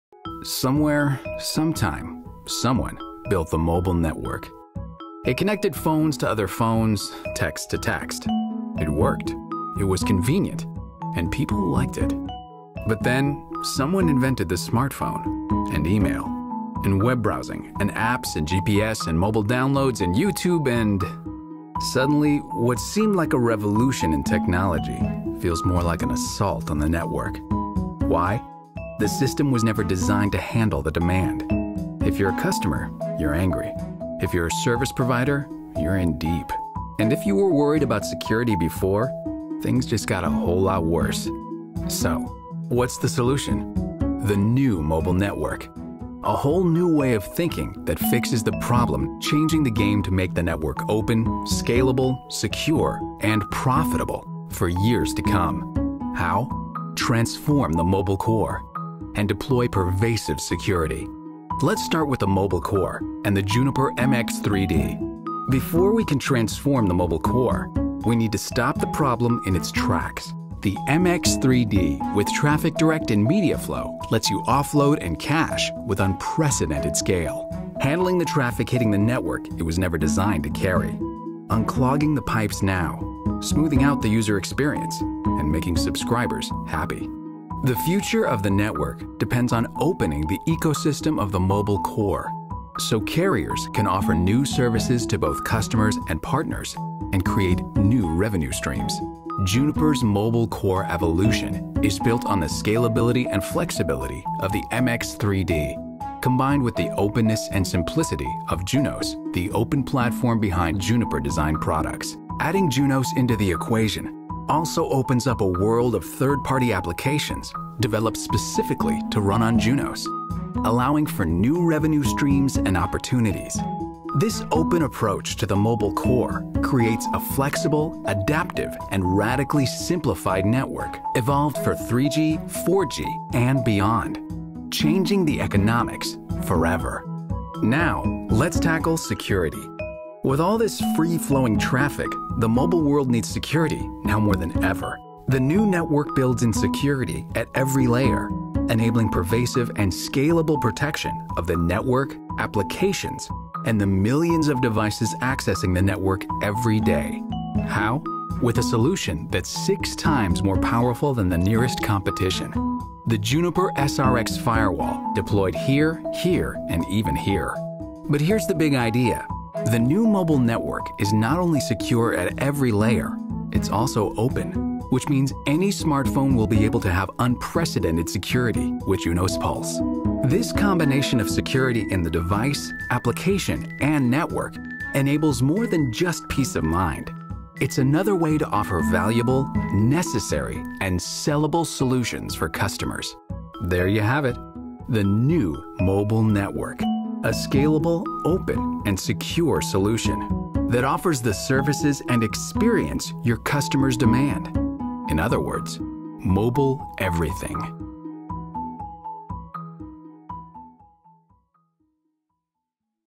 Juniper Networks Mobile Core Evolution
The mobile network is suffering from an explosion of flat-rate data traffic.  See how Juniper Networks can transform the economics of the network for 3G, 4G and beyond.